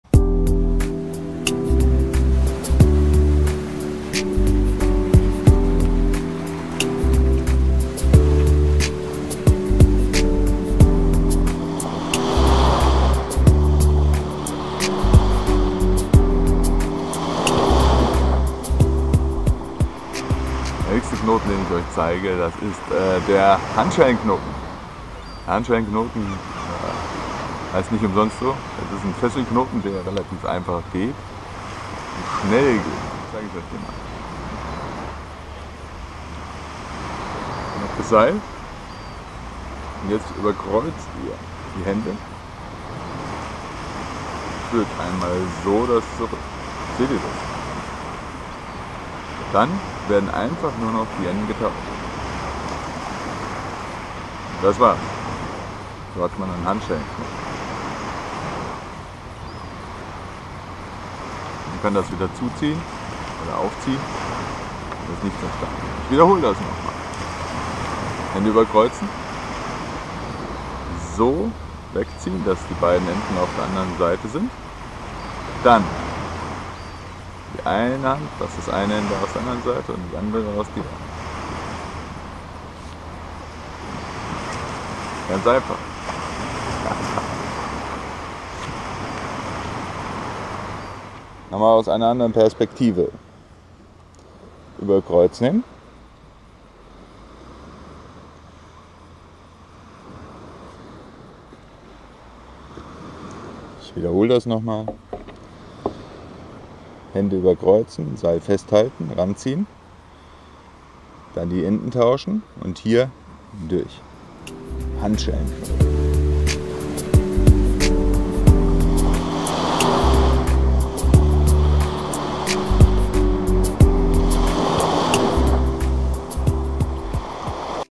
Handschellenknoten - ein Fesselknoten nicht nur für die Polizei gedacht
Der Fesselknoten oder auch Handschellenknoten ist ein Verwandter des Webleinstegs. Er hat zwei lose Augen mit denen man nicht nur Gliedmaße fesseln kann, sondern auch einfach z.B. Wanderstöcke am Rucksack befestigen kann. Also ein Knoten auch fürs wandern oder bushcraften. Viel Spaß damit.